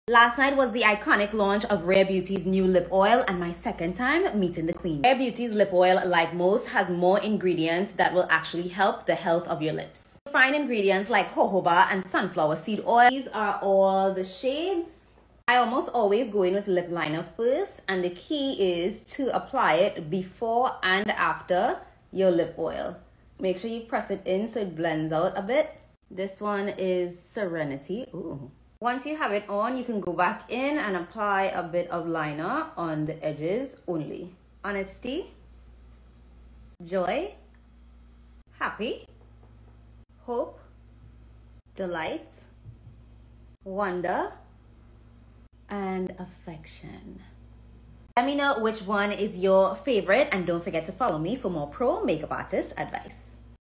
TRYING ALL THE RARE BEAUTY TINTED LIP OILS!!
Trying ALL the shades of the NEW RareBeauty Soft Pinch Tinted Lip Oil 💕 of course have to show you my tip on how to make it last.If you’re wondering the difference between a lip oil & a lip…hunnie lemme show you! Thank you so much

LISTEN TO MY BEAUTY PODCAST!